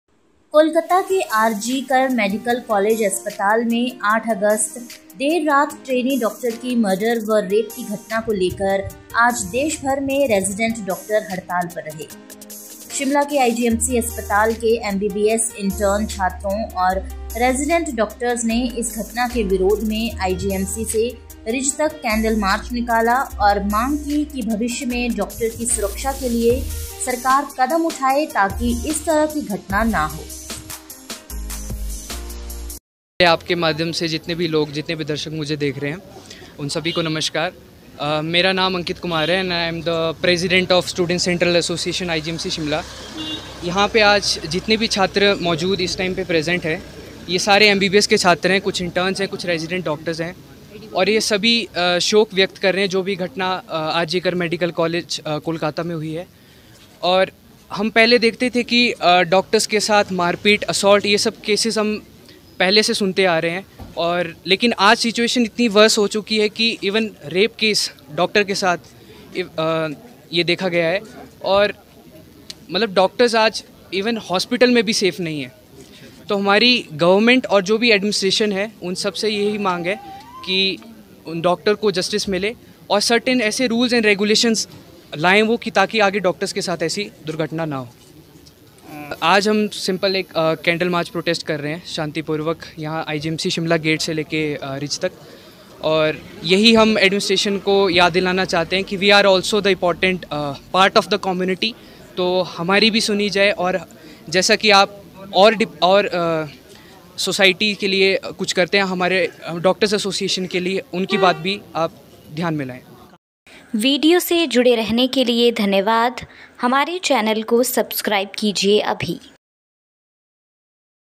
कोलकाता डॉक्टर केस के विरोध में शिमला में रेजिडेंट डॉक्टर्स ने निकाला कैंडल मार्च
कोलकाता के आरजी कर मेडिकल कॉलेज अस्पताल में 8 अगस्त देर रात ट्रेनी डॉक्टर की मर्डर व रेप की घटना को लेकर आज देशभर में रेजिडेंट डॉक्टर हड़ताल पर रहे। शिमला के आईजीएमसी अस्पताल के एमबीबीएस इंटर्न छात्रों और रेजिडेंट डॉक्टर्स ने भी घटना के विरोध में  ICMC से रिज तक कैंडल मार्च निकाला और मांग की है कि भविष्य में डॉक्टर की सुरक्षा  के लिए सरकार कदम उठाए ताकि इस तरह की घटना न हो।